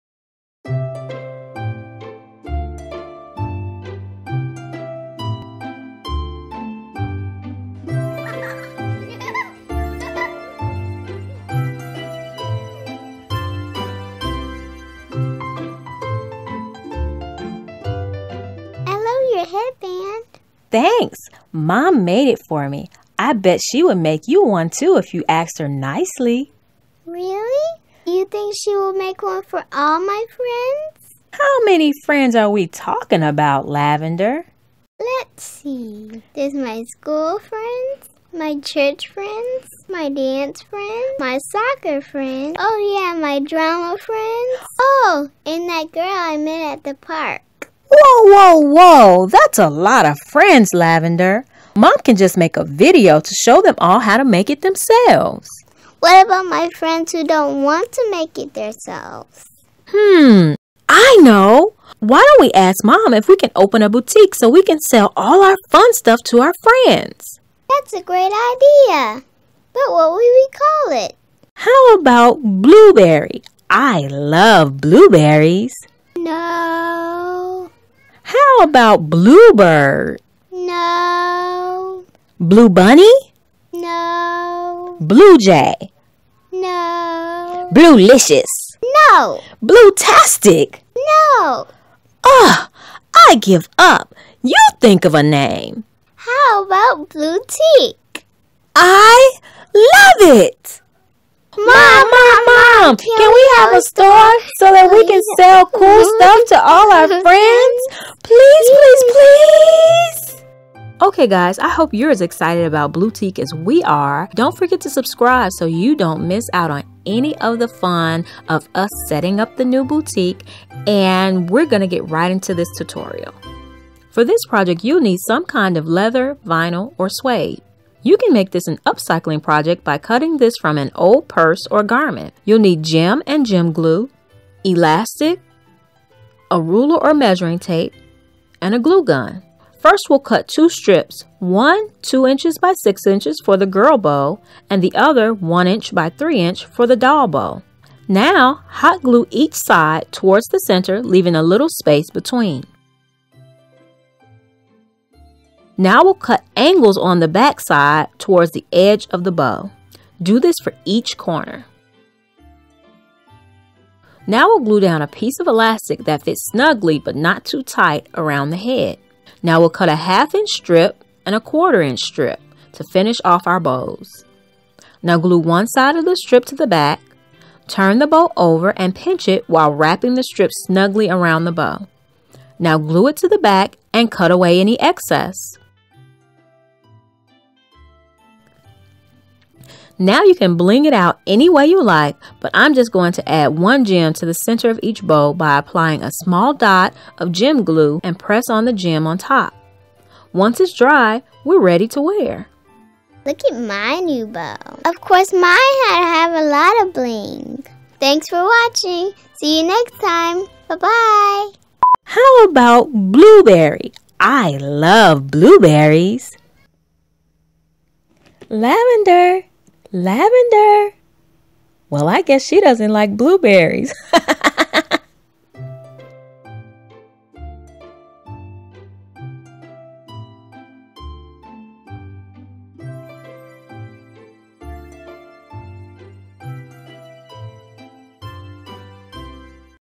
Make Bow Headbands for Girls & Dolls | Baby Alive & AG type dolls | Blueprint DIY Kids
Let's make matching bow headbands for our dolls! I made mine with some leather from an old skirt. It doesn't have to be real leather of course. It could definitely be faux leather or suede. This is an easy no-sew project, so have fun!

For this project you'll need:
SCISSORS
LEATHER, VINYL, OR SUEDE (does not have to be real)
RULER
ELASTIC
GLUE GUN
GEMS
GEM GLUE

This project is great for Baby Alive, American Girl, Reborn, Our Generation, Cabbage Patch, Build-a-Bear, or any 15 to 18" doll.

+++ DOLL DETAILS +++

LAVENDER Baby Alive Baby's New Teeth - Thrifted Goodwill
CRISYNTHAMA Baby Alive Baby All Gone (pees & poops) - Walmart
DAISY Baby Alive Better Now Baby - Value Village
HANNAH Springfield Collection Sofia - Joann Fabrics
LILYANA Springfield Collection Emma - Joann Fabrics
ROSE Springfield Collection Madison- Joann Fabrics

+++ MUSIC +++